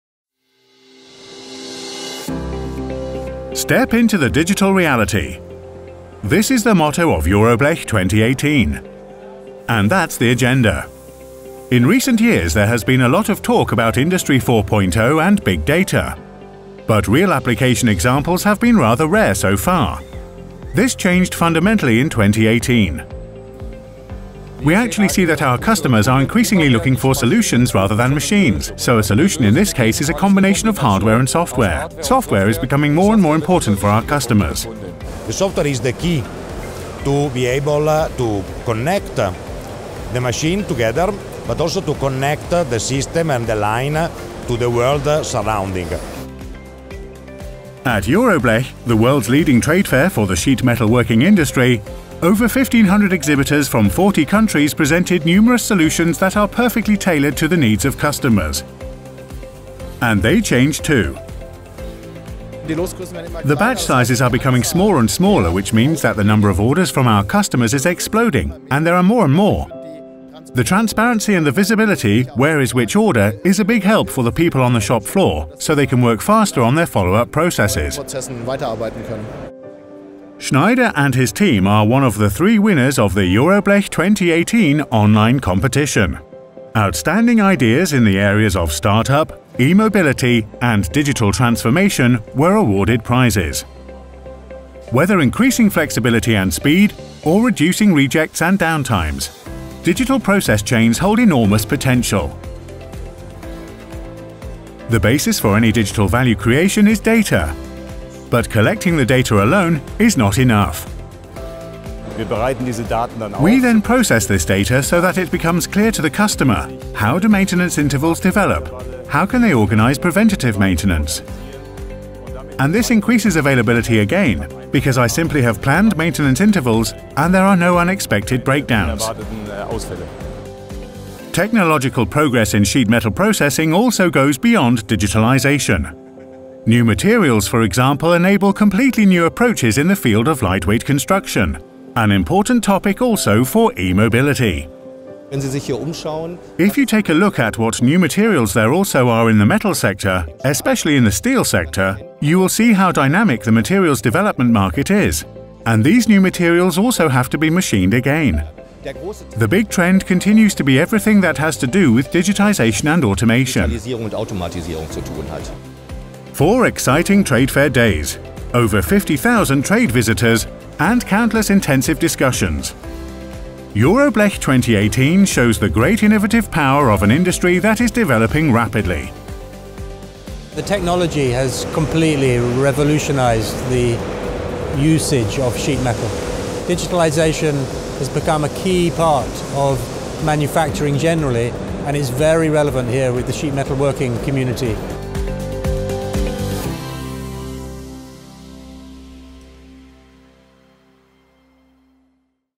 EuroBLECH 2018 - Step into the digital reality
This year's EuroBLECH, which took place from 23 to 26 October 2018 in Hanover, Germany, was all about digitalisation, automation and smart manufacturing in the sheet metal working industry.